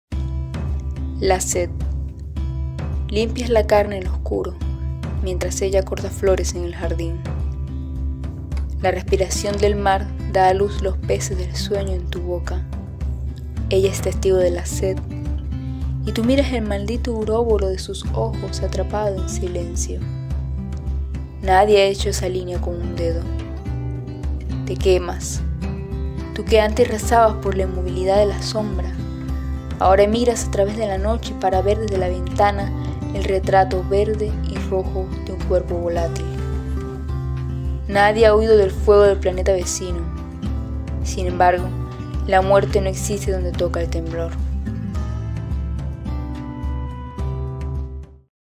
La sed, de Paola Valencia
Videopoema inspirado en el texto de la autora venezolana Paola Valencia (1997).
Realizado por Gladys Mendía
Colección Clips Modernos: Audiovisuales de la poesía venezolana
Santiago de Chile, febrero 2021.